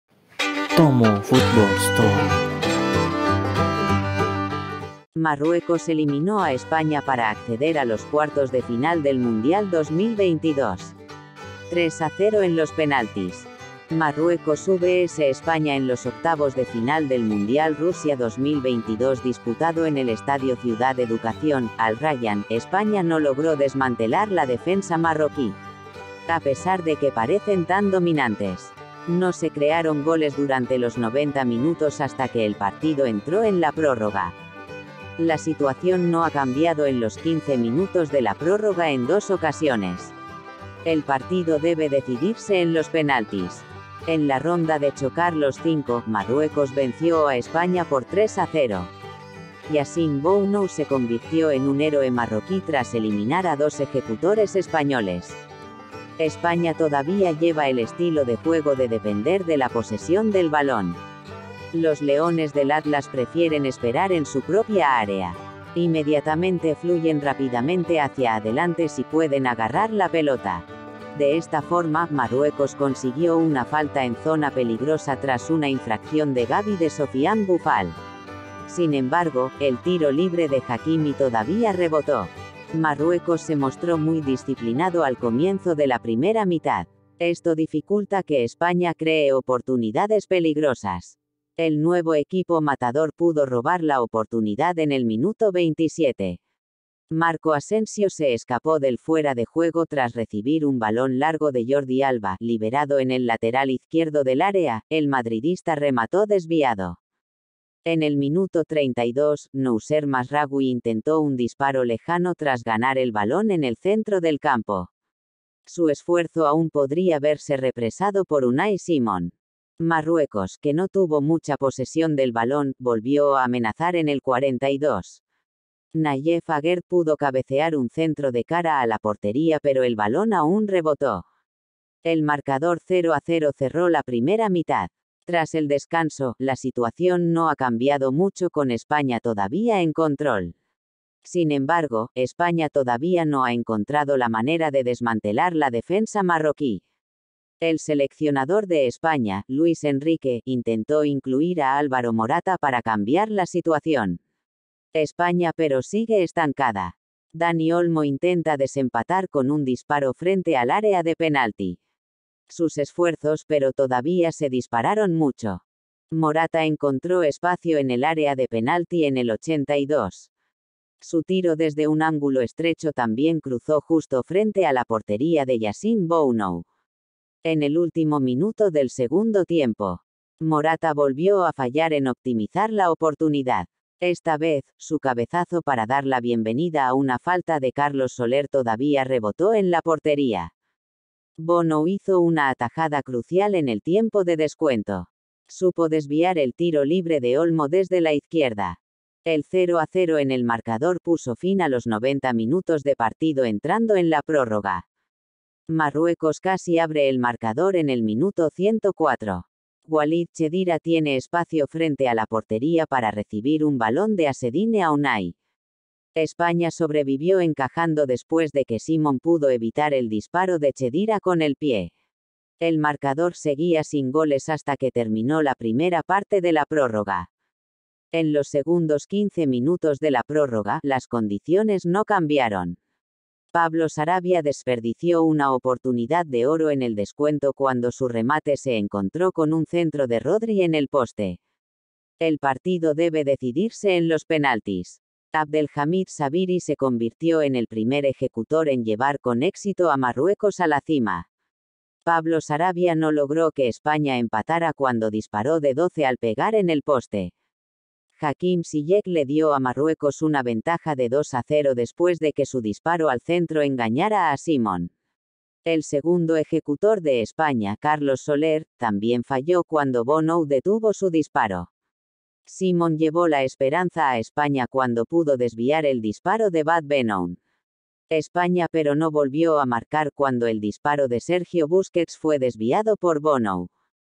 RESUMEN DESTACADOS España vs marruecos 0-0 Marruecos dio la sorpresa
Marruecos eliminó a España para acceder a los cuartos de final del Mundial 2022. 3 a 0 en los penaltis.

  Marruecos vs España en los octavos de final del Mundial Rusia 2022 disputado en el Estadio Ciudad Educación, Al Rayyan, España no logró desmantelar la defensa marroquí

  A pesar de que parecen tan dominantes.  No se crearon goles durante los 90 minutos hasta que el partido entró en la prórroga.

  La situación no ha cambiado en los 15 minutos de la prórroga en dos ocasiones.  El partido debe decidirse en los penaltis.

  En la ronda de chocar los cinco, Marruecos venció a España por 3 a 0. Yassine Bounou se convirtió en un héroe marroquí tras eliminar a dos ejecutores españoles.

  España todavía lleva el estilo de juego de depender de la posesión del balón.  Los leones del Atlas prefieren esperar en su propia área.  Inmediatamente fluyen rápidamente hacia adelante si pueden agarrar la pelota.

  De esta forma, Marruecos consiguió una falta en zona peligrosa tras una infracción de Gavi de Sofiane Boufal.  Sin embargo, el tiro libre de Hakimi todavía rebotó.

  Marruecos se mostró muy disciplinado al comienzo de la primera mitad.  Esto dificulta que España cree oportunidades peligrosas.

  El nuevo equipo Matador pudo robar la oportunidad en el minuto 27.  Marco Asensio se escapó del fuera de juego tras recibir un balón largo de Jordi Alba, liberado en el lateral izquierdo del área, el madridista remató desviado.

  En el minuto 32, Noussair Mazraoui intentó un disparo lejano tras ganar el balón en el centro del campo.  Su esfuerzo aún podría verse represado por Unai Simon.

  Marruecos, que no tuvo mucha posesión del balón, volvió a amenazar en el 42'.  Nayef Aguerd pudo cabecear un centro de cara a la portería pero el balón aún rebotó.

  El marcador 0-0 cerró la primera mitad.

  Tras el descanso, la situación no ha cambiado mucho con España todavía en control.  Sin embargo, España todavía no ha encontrado la manera de desmantelar la defensa marroquí.

  El seleccionador de España, Luis Enrique, intentó incluir a Álvaro Morata para cambiar la situación.  España pero sigue estancada.

  Dani Olmo intenta desempatar con un disparo frente al área de penalti.  Sus esfuerzos, pero todavía se dispararon mucho.

  Morata encontró espacio en el área de penalti en el 82'.  Su tiro desde un ángulo estrecho también cruzó justo frente a la portería de Yassine Bounou.

  En el último minuto del segundo tiempo.  Morata volvió a fallar en optimizar la oportunidad.  Esta vez, su cabezazo para dar la bienvenida a una falta de Carlos Soler todavía rebotó en la portería.

  El 0-0 en el marcador puso fin a los 90 minutos de partido entrando en la prórroga.

  Marruecos casi abre el marcador en el minuto 104.  Walid Cheddira tiene espacio frente a la portería para recibir un balón de Azzedine Ounahi.

  España sobrevivió encajando después de que Simon pudo evitar el disparo de Cheddira con el pie.  El marcador seguía sin goles hasta que terminó la primera parte de la prórroga.

  En los segundos 15 minutos de la prórroga, las condiciones no cambiaron.  Pablo Sarabia desperdició una oportunidad de oro en el descuento cuando su remate se encontró con un centro de Rodri en el poste.

  El partido debe decidirse en los penaltis.

  Abdelhamid Sabiri se convirtió en el primer ejecutor en llevar con éxito a Marruecos a la cima.  Pablo Sarabia no logró que España empatara cuando disparó de 12 al pegar en el poste.

  Hakim Ziyech le dio a Marruecos una ventaja de 2-0 después de que su disparo al centro engañara a Simon.  El segundo ejecutor de España, Carlos Soler, también falló cuando Bonou detuvo su disparo.

  Simon llevó la esperanza a España cuando pudo desviar el disparo de Badr Benoun.  España pero no volvió a marcar cuando el disparo de Sergio Busquets fue desviado por Bonou.

  Hakimi se aseguró el pase de Marruecos a cuartos de final cuando su cosecha venció a Simon.  Marruecos ganó 3-0.